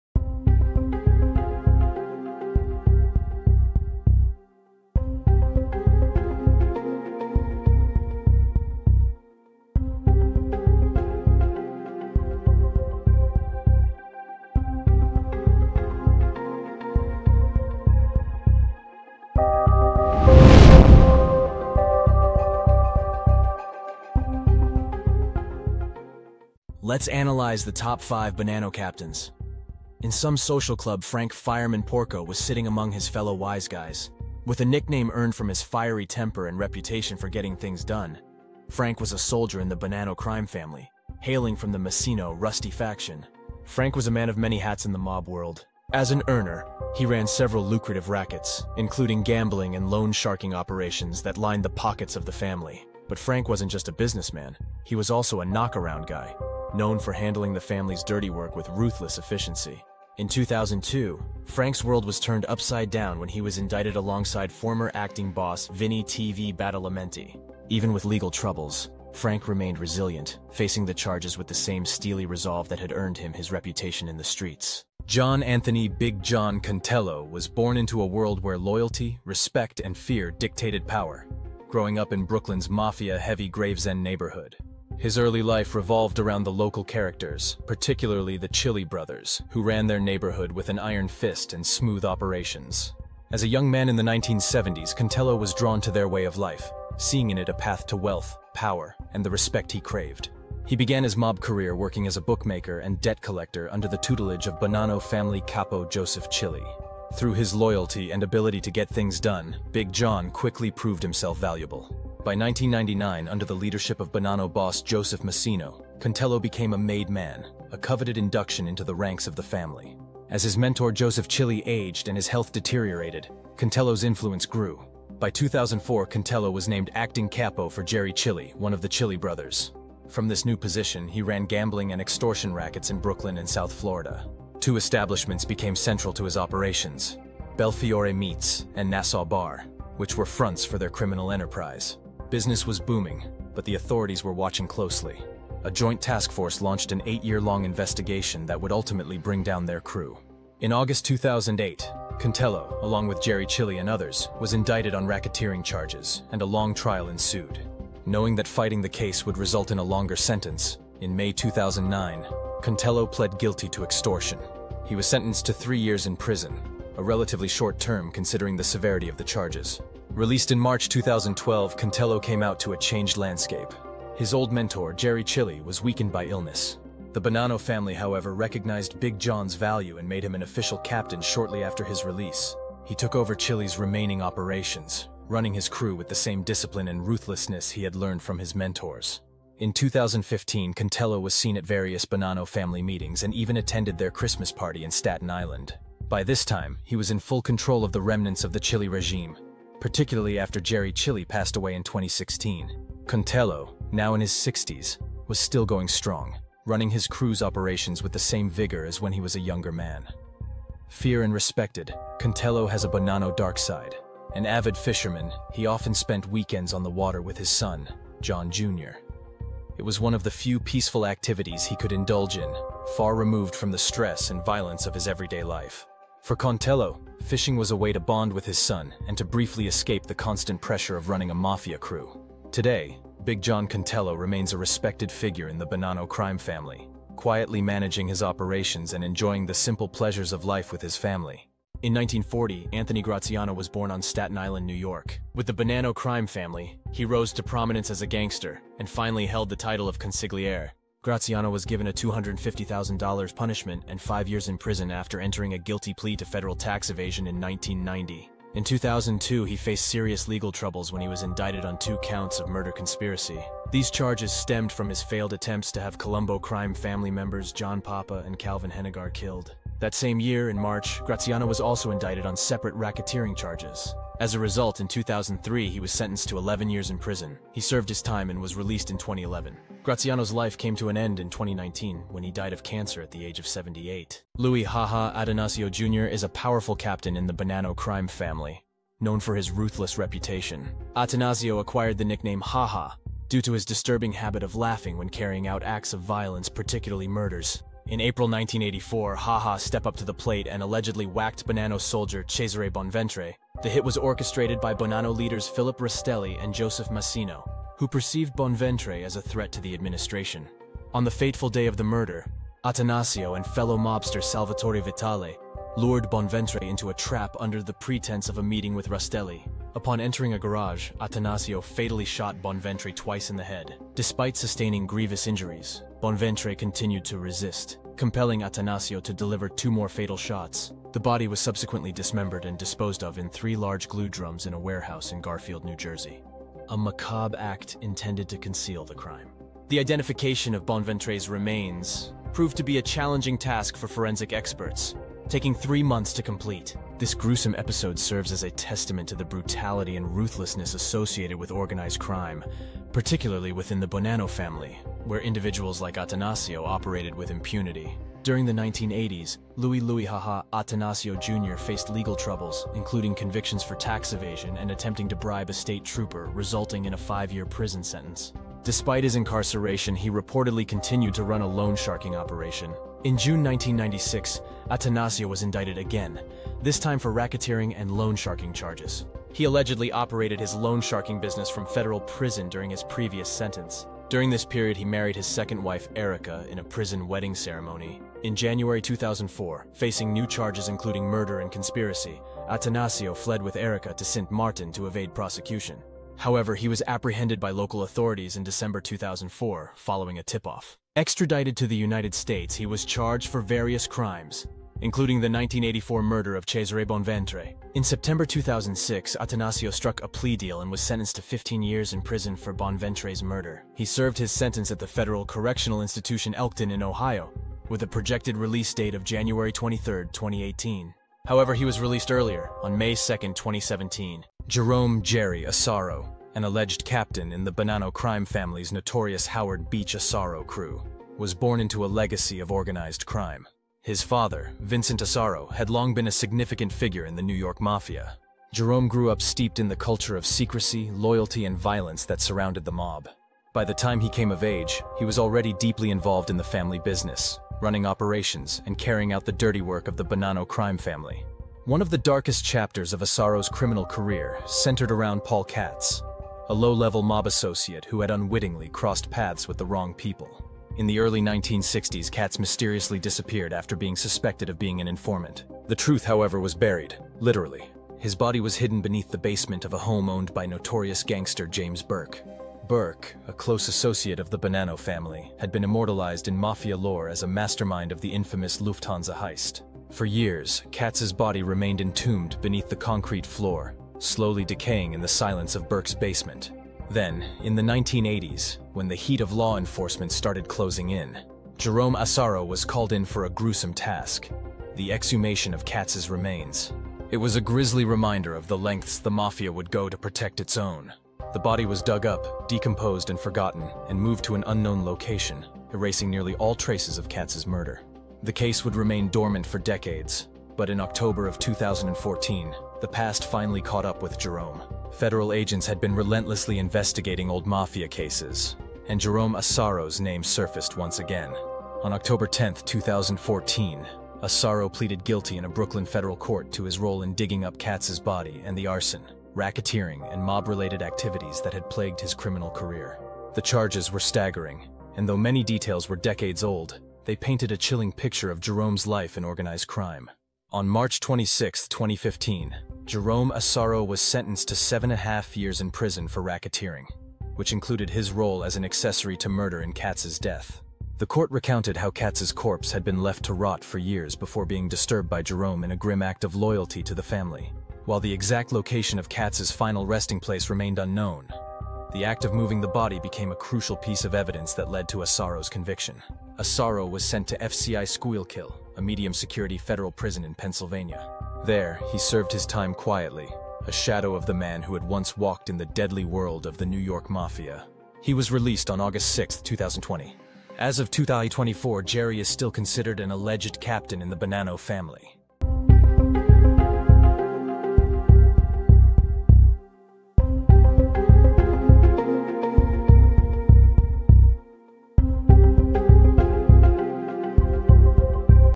PATREON EXCLUSIVE: TOP 5 BONANNO CAPTAINS
In this video, we check out the infamous Bonanno crime family, focusing on five of their most powerful and notorious captains and past and present. From the ruthless Louie Attanasio to veteran mobster Anthony Graziano, and the influential John Contello, we explore the lives, crimes, and legacies of these feared Mob figures. Discover how they rose through the ranks, the key roles they played in the family's operations, and the lasting impact they've had on the New York Cosa Nostra. Don't miss this deep dive into organized crime's elite!